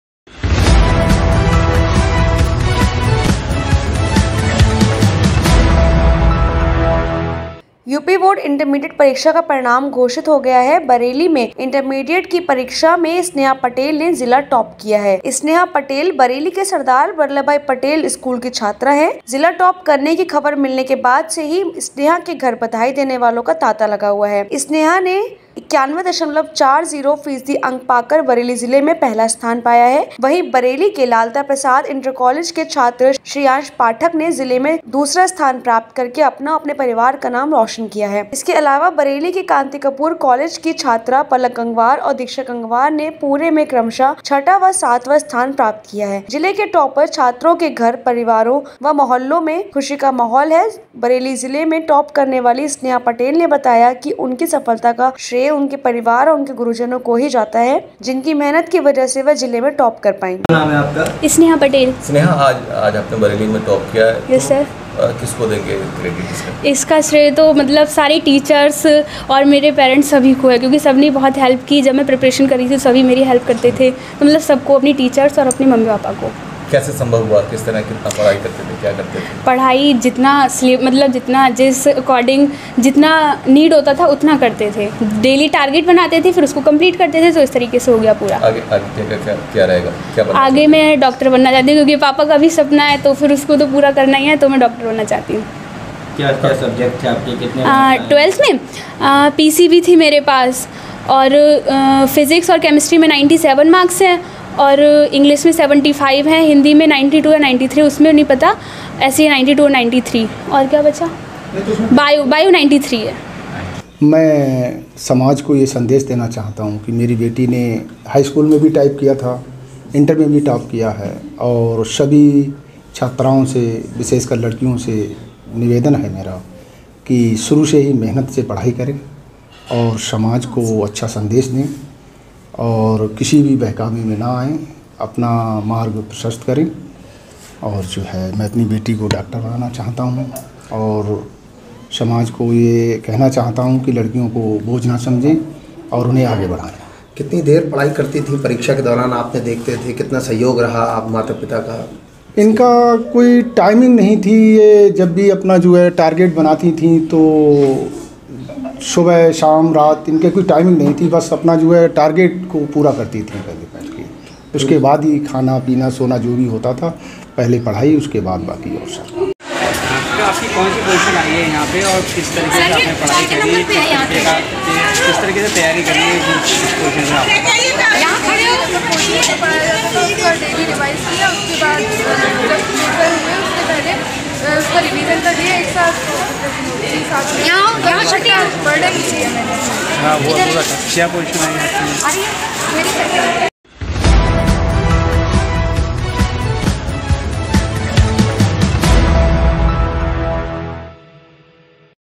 इंटरमीडिट एग्जाम में स्नेहा पटेल ने बरेली में किया टॉप
यूपी बोर्ड इंटरमीडिएट परीक्षा का परिणाम घोषित हो गया है। बरेली में इंटरमीडिएट की परीक्षा की स्नेहा पटेल ने जिला टाप किया है। स्नेहा पटेल बरेली के सरदार वल्लभ भाई पटेल स्कूल की छात्रा हैं। जिला टाप करने की खबर मिलने के बाद से ही स्नेहा के घर बधाई देने वालों का तांता लगा हुआ है। स्नेहा पटेल ने 91.40 फीसदी अंक पाकर बरेली जिले में पहला स्थान पाया है। वहीं बरेली के लालता प्रसाद इंटर कालेज के छात्र श्रीयांश पाठक ने जिले में दूसरा स्थान प्राप्त करके अपना और परिवार का नाम रोशन किया है। श्रीयांश पाठक ने 91.20 फीसदी अंक प्राप्त किए हैं। इसके अलावा बरेली के कांति कपूर इंटर कालेज की छात्रा पलक गंगवार और दीक्षा गंगवार ने पूरे में क्रमशः छठवां और सातवां स्थान प्राप्त किया है।जिले के टापर छात्रों के घरों परिवारों व मोहल्लों में ख़ुशी का माहौल है। बरेली जिले में टाप करने वाली स्नेहा पटेल ने बताया कि उसकी इस सफलता का श्रेय उसके परिवार और उसके गुरूजनों को ही जाता है। स्नेहा ने कहा कि मेरी इस सफलता के पीछे से सबसे पहला हाथ मेरे गुरूजनों का है जिनकी मेहनत की वजह से वह जिले में टाप कर पायी। वहीं परिवार का भी परीक्षा के दौरान पूरा सहयोग मिला है, जिससे उसको यह सफलता मिली है।

 UPBreakingnews UttarPradesh
uttarpradeshnews
lucknow
upnews
uttarpradeshnewslive
UPhindinews
uttarpradeshgovernment
Uttarpradeshpolice
CMYogiAditiyanath